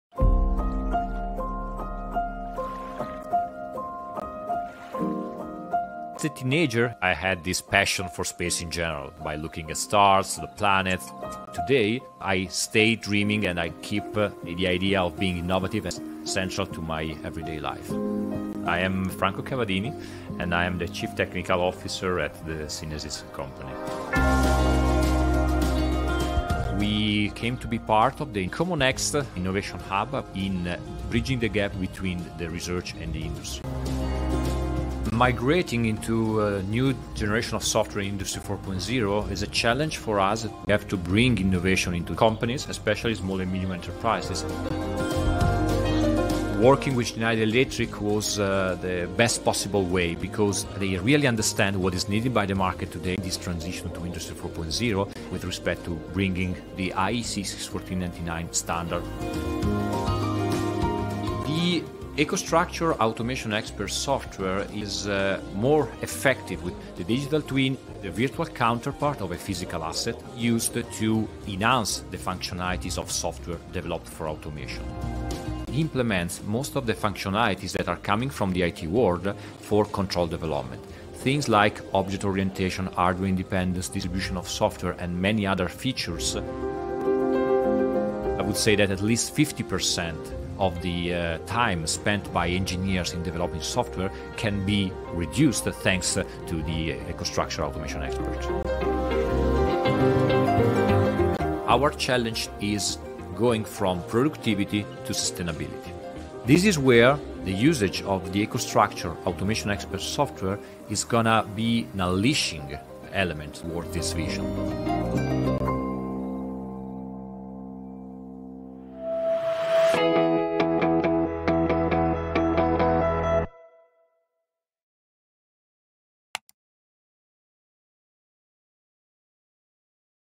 Câu Chuyện Thành Công của Synesis với EcoStruxure Automation Expert | Schneider Electric Việt Nam
Hãy khám phá cách mà Synesis áp dụng những đổi mới vào cuộc sống với EcoStruxure Automation Expert, hệ thống phần mềm tự động hóa công nghiệp của Schneider Electric, không giống như bất kỳ thứ gì bạn đã thấy trước đây.

EcoStruxure Automation Expert hỗ trợ kiến trúc tự động hóa linh hoạt, tập trung vào thiết bị, giúp hiệu suất có thể được kiểm soát trong toàn bộ vòng đời tự động hóa để mang lại những cải tiến thực sự, có thể đo đếm được cho tất cả các biến số kinh doanh, đặc biệt là khả năng sinh lời.

Video này nói về cách mà phần mềm EcoStruxure Automation Expert của Schneider Electric mang đến sự cải tiến cho Synesis.